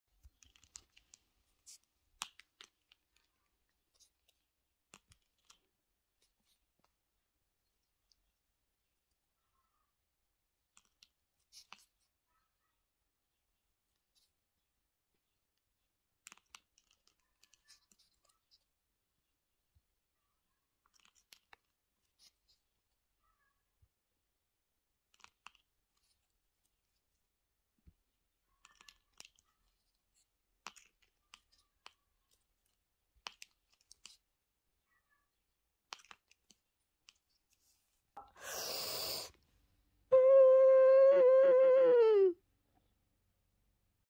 Terry doesn't let's Me to Take My Vape
Terry: Smoking kills, i won't give it!
Me: I'm not smoking, I'm vaping...